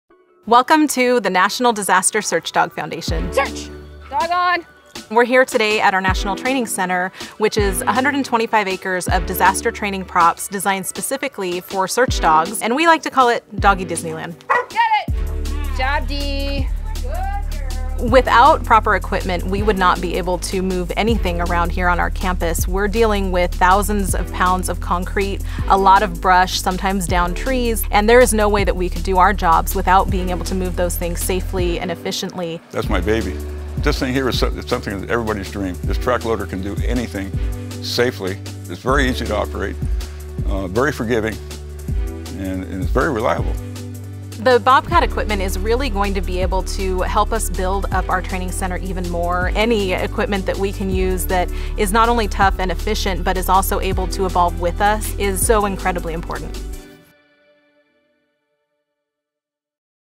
Bobcat Equipment Used for Disaster Simulation Sites for Search Dog Foundation
The National Disaster Search Dog Foundation relies on Bobcat® equipment every day to support their rescue training efforts. This non-profit organization trains search and rescue dogs to search for missing persons in disasters around the country. Their campus in Santa Paula, California, includes offices, housing, veterinary facilities, and several miles of simulated disaster sites used for realistic search dog training, and Bobcat equipment is there to support it all.